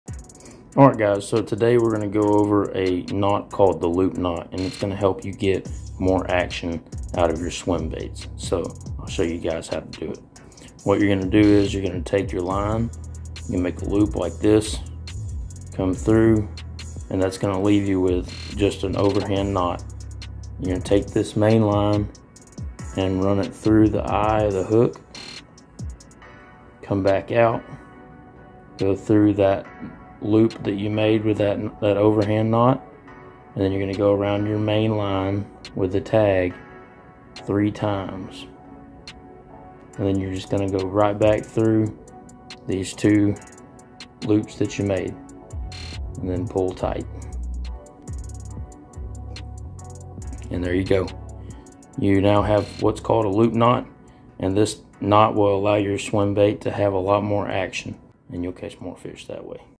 How To Tie a Loop Knot To Maximize Action in a Swimbait
This Knot will help you get the most out of your swimbaits, and catch for fish than your buddy tying a Palomar knot.